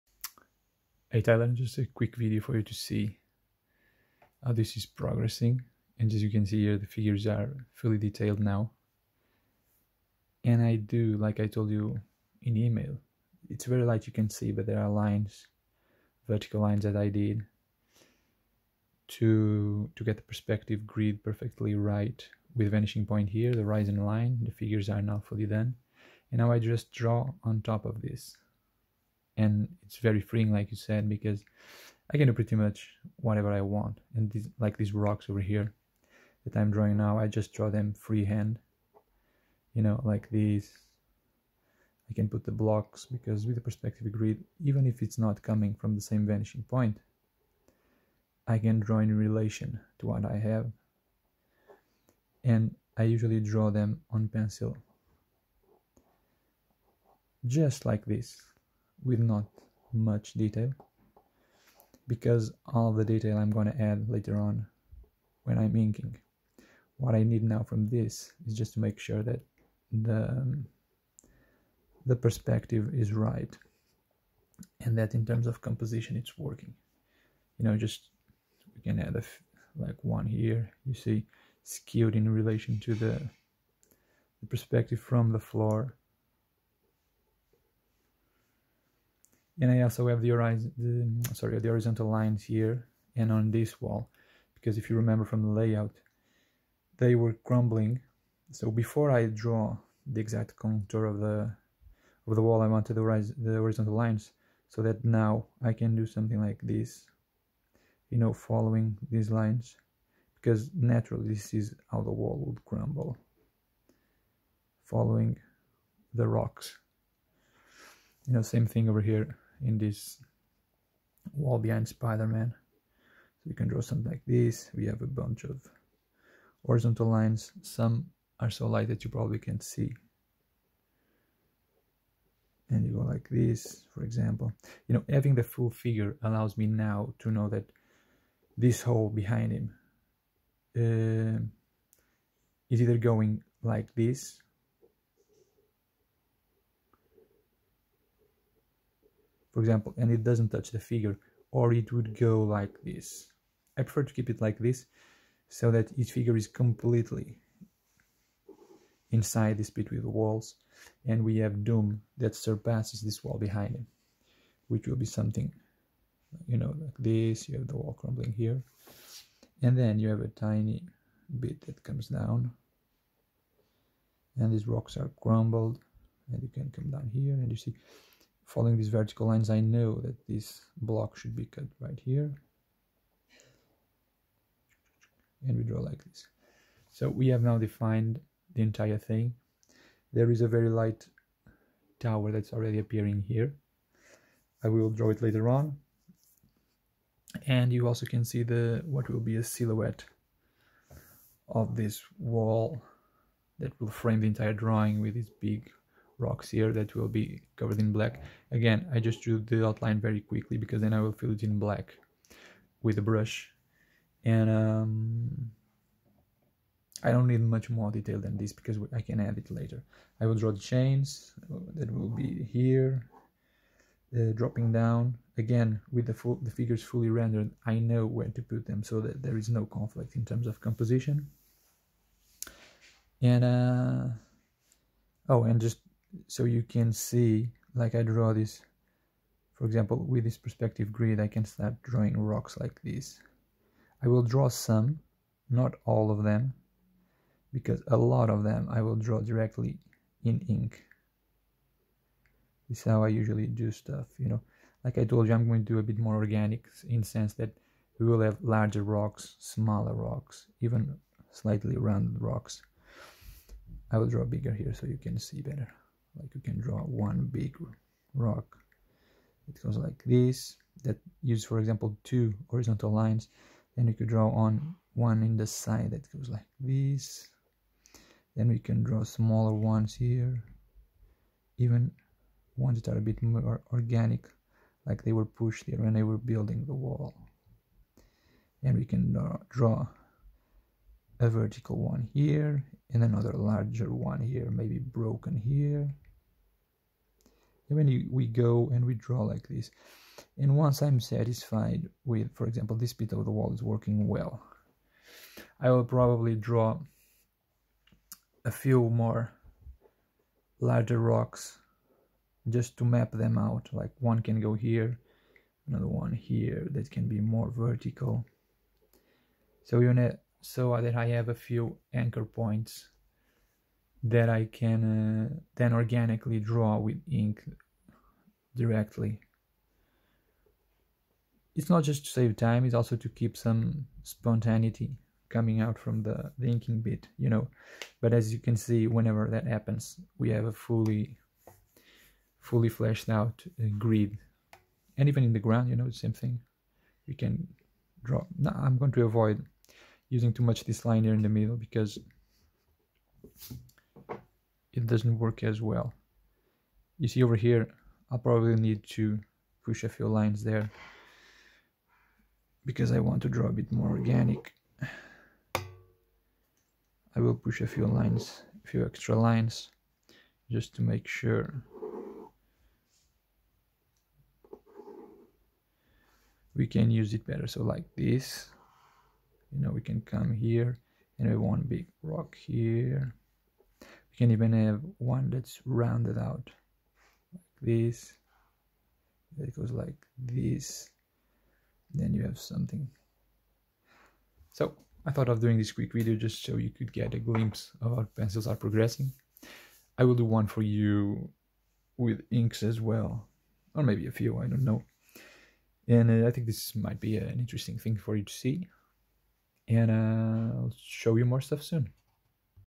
Spider-Man + Doom commission - Process talk
Video sent to the client of this commission, explaining the process of penciling and laying details over a perspective grid.